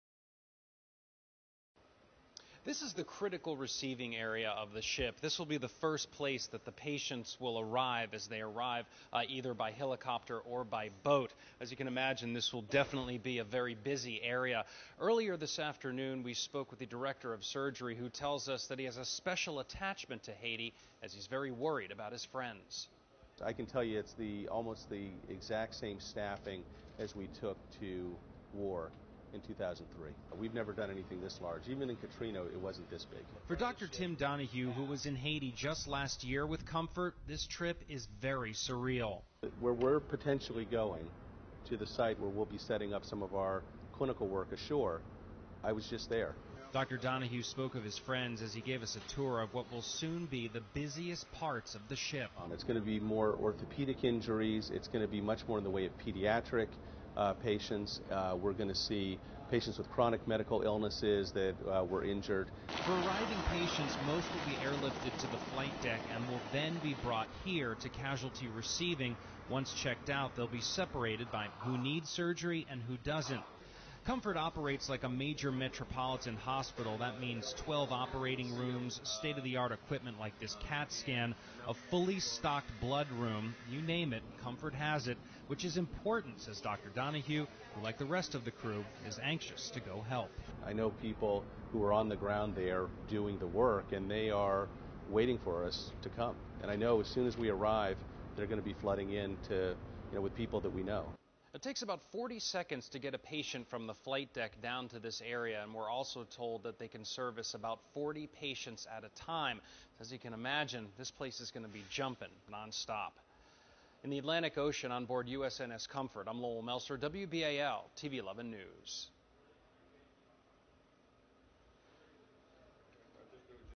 Comfort's Director Of Surgery Talks To Lowell
11 News reporter Lowell Melser reports aboard the USNS Comfort.